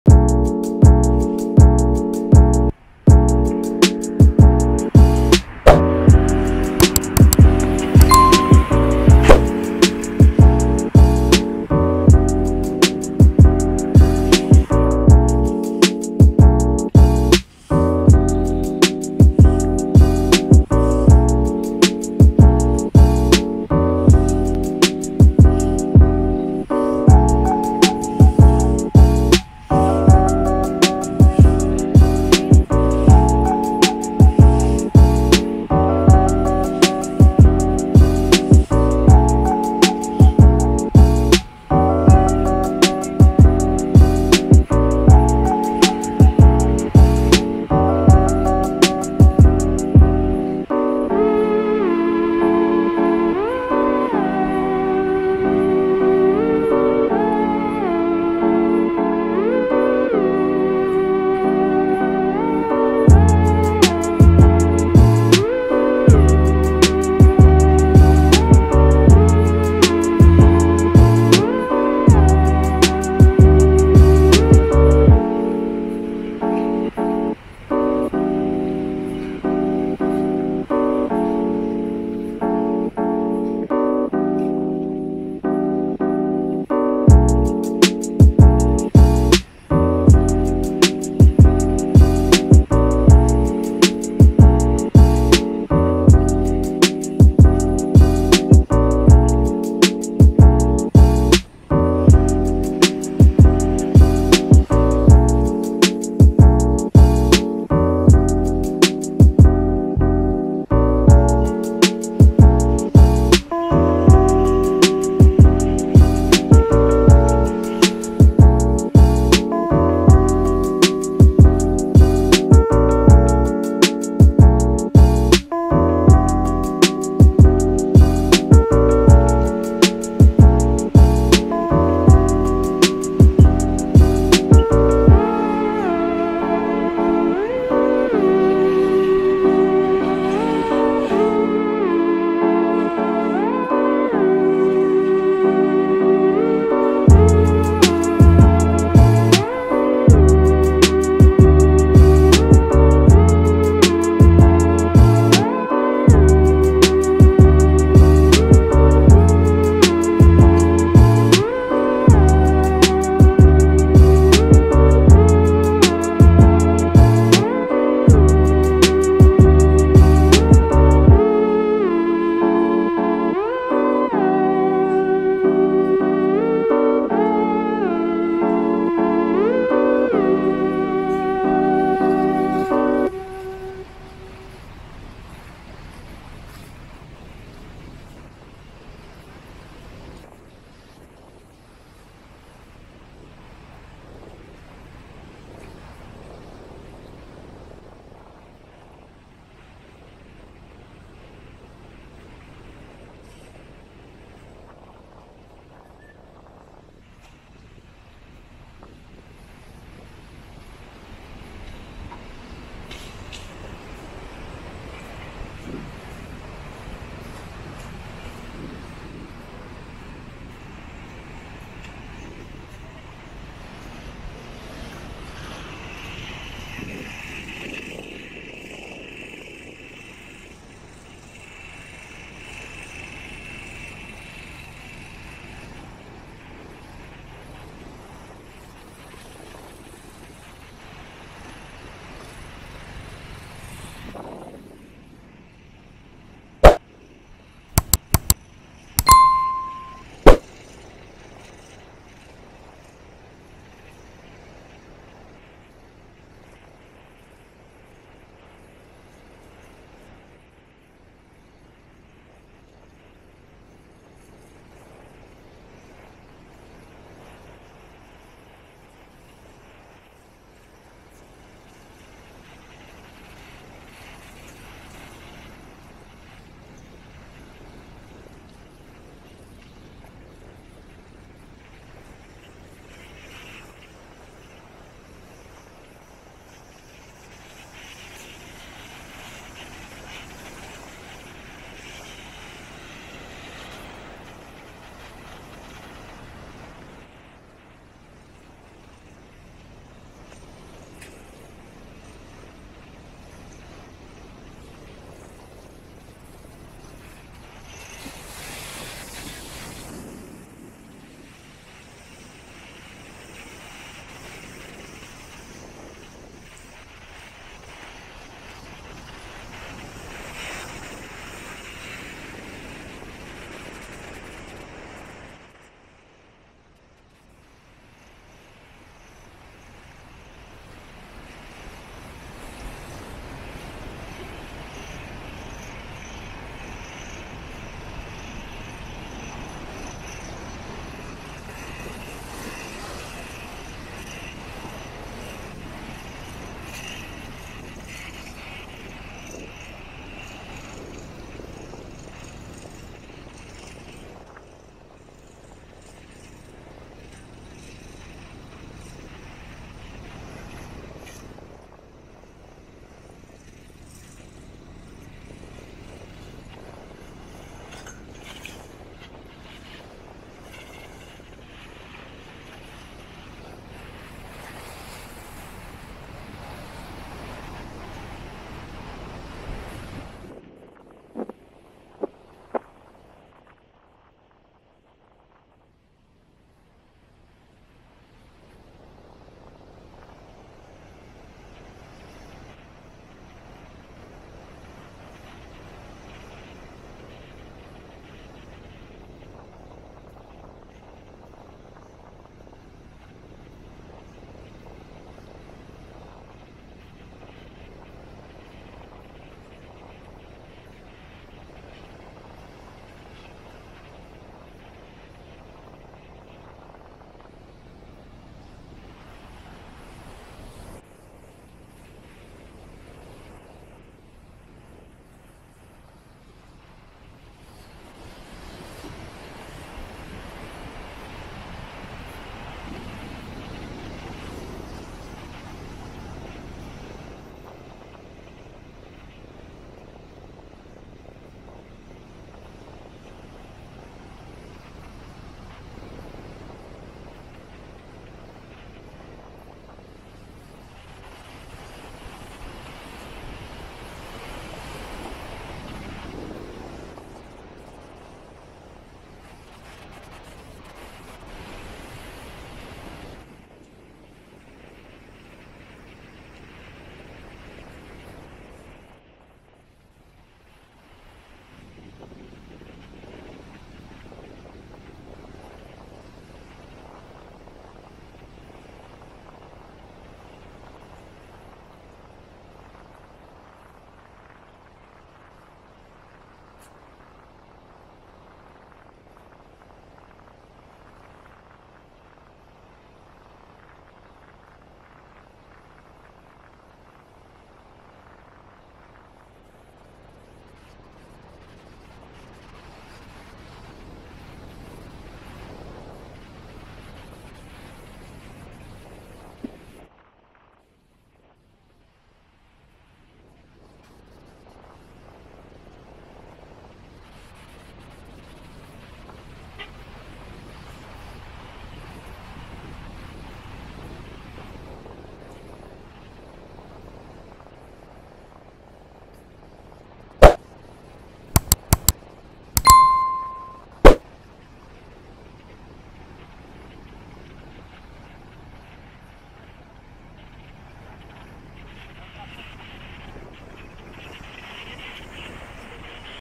☀️GRAN CANARIA ☀️ Natural Pools Walk November 2020🌴 Piscinas Naturales de Castillo del Romeral 😎
In Today's Video we take a walk through the natural pools of Castillo del Romeral A Very Pleasant Place..